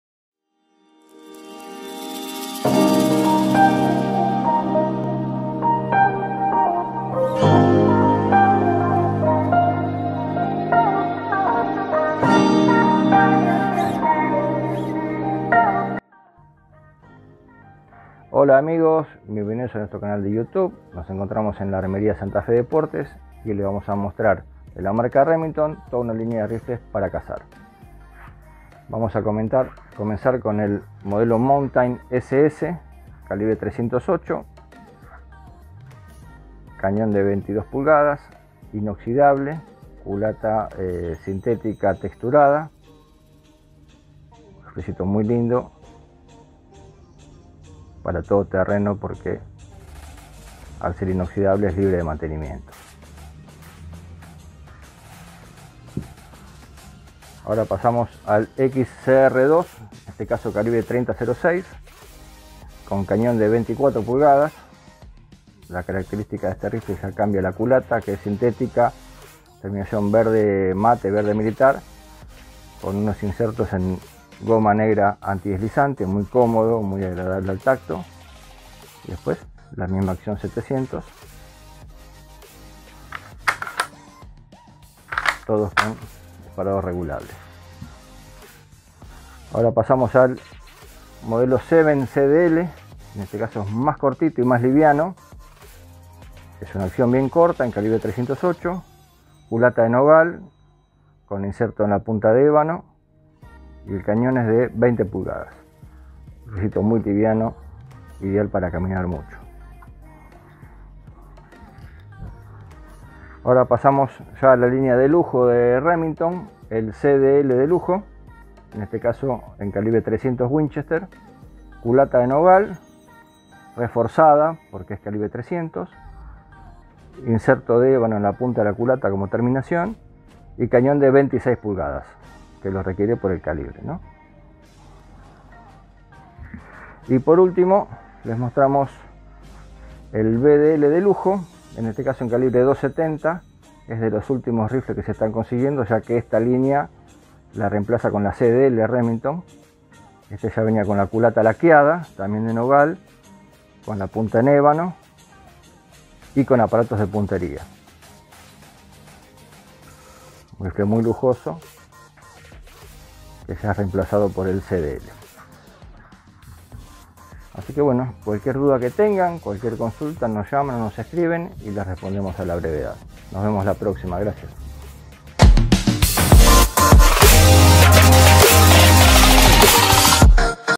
Fusiles Remington, línea de caza
No te pierdas en este video una completa variedad de fusiles de caza, de la marca Remington. Su exclusivo diseño y excelente funcionamiento hacen que esta marca sea una de las más prestigiosas del mundo.